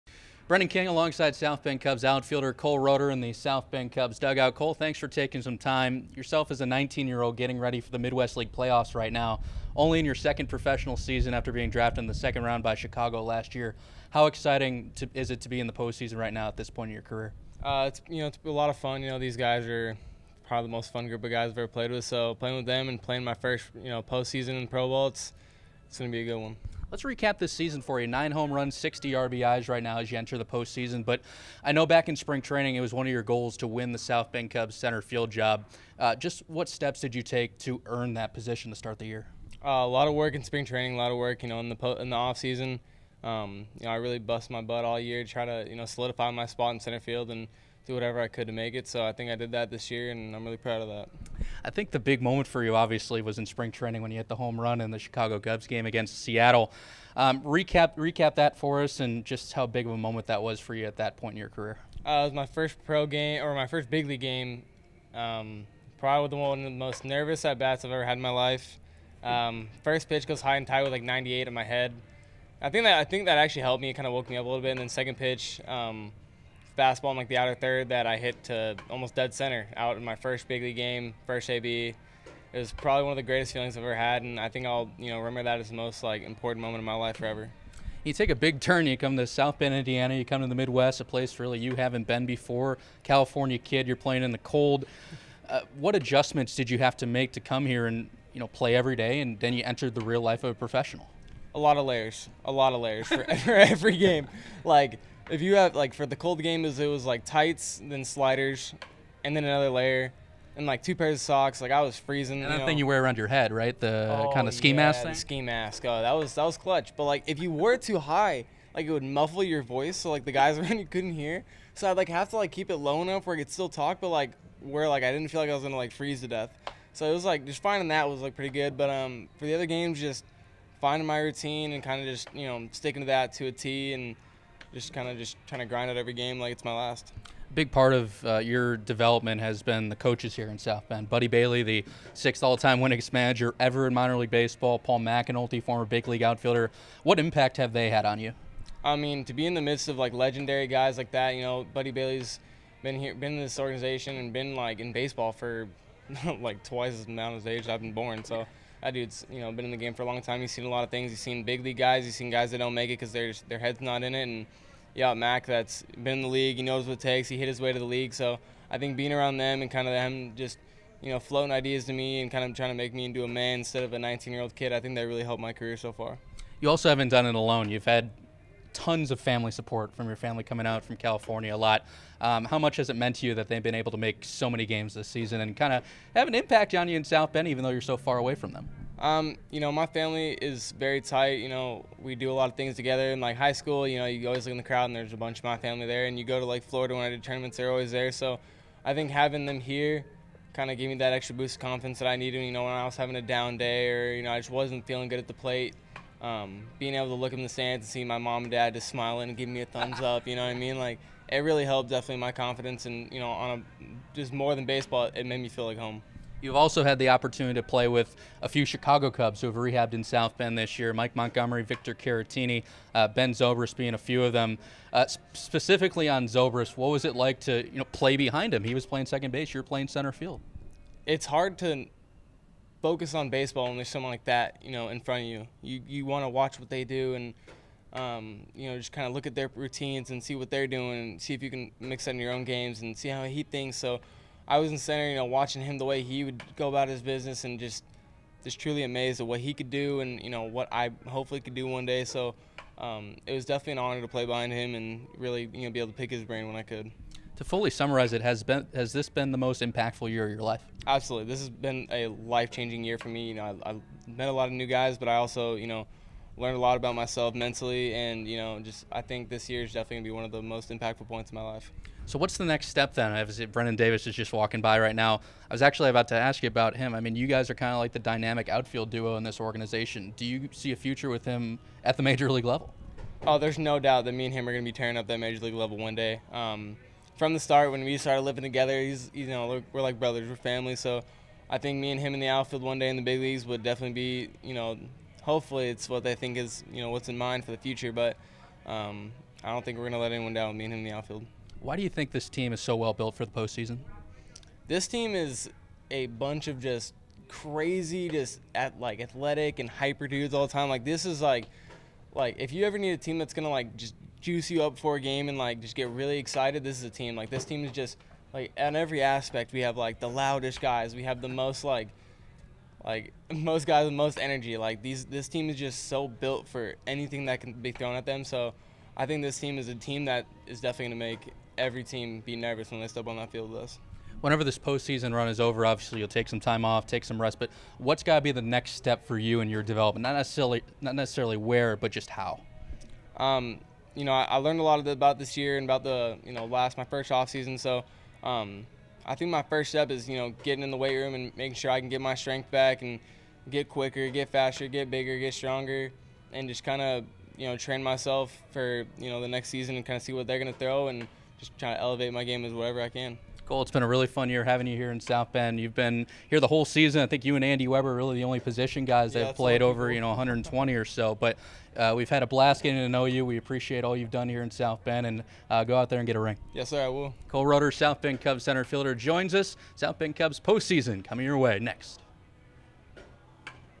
Inside the Clubhouse - Cole Roederer
Before round two of the Midwest League Postseason kicks off, South Bend Cubs Broadcaster Brendan King sat down with Outfielder Cole Roederer to preview the team's playoff run this month. The guys also discussed Cole's full 2019 season and what his next step is in the Chicago Cubs organization.